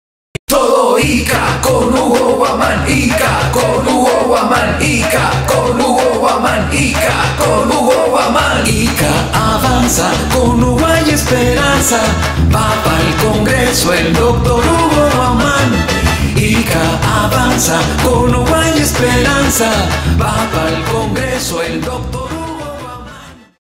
Animación electoral 2 - Dr. Hugo Huamán Brizuela Campaña congresal Ica 2020
Este es uno de nuestros múltiples trabajos realizados para el candidato al congreso 2020 Dr. Hugo Huamán Brizuela. Trabajamos con compromiso y profesionalismo, somos 5 online, visión de futuro. 🧐😃